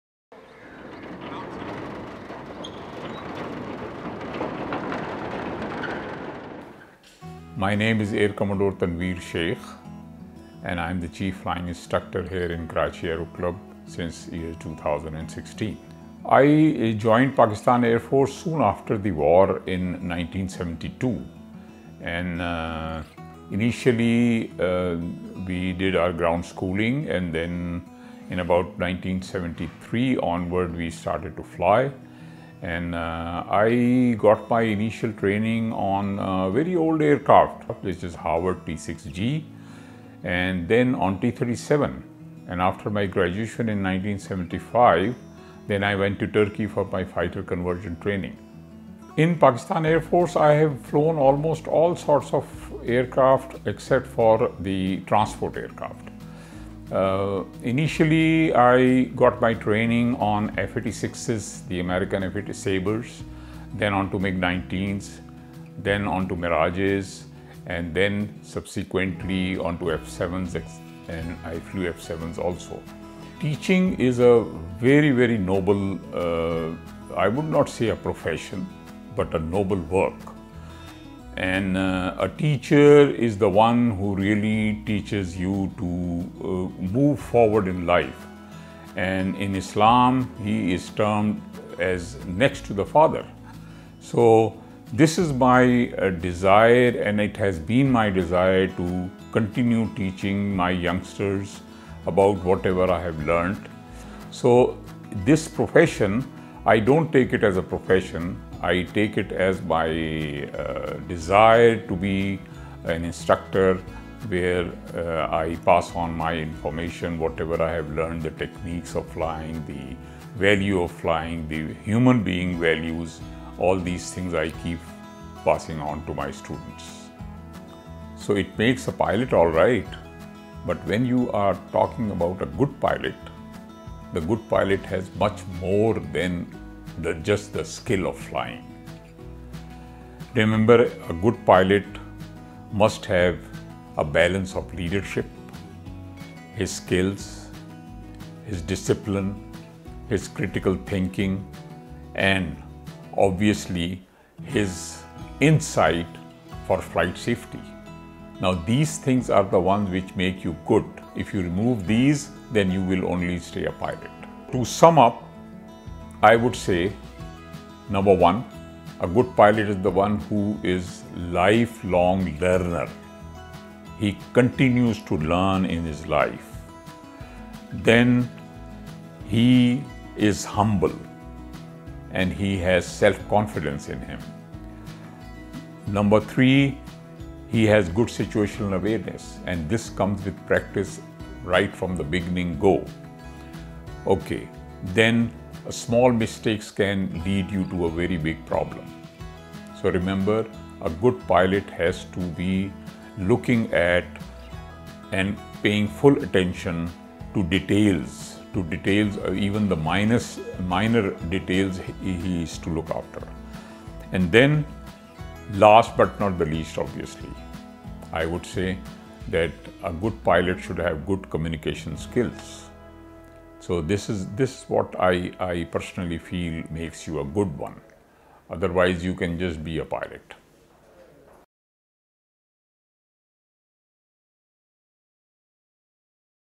Meet the Chief Flying Instructor - Air Commodore (R) Tanweer M. Sheikh
Watch the profile video on our Chief Flying Instructor, Air Commodore (R) Tanweer M. Sheikh as he talks about his experience flying with the Pakistan Air Force, why he loves teaching and what traits he thinks make for a good Pilot.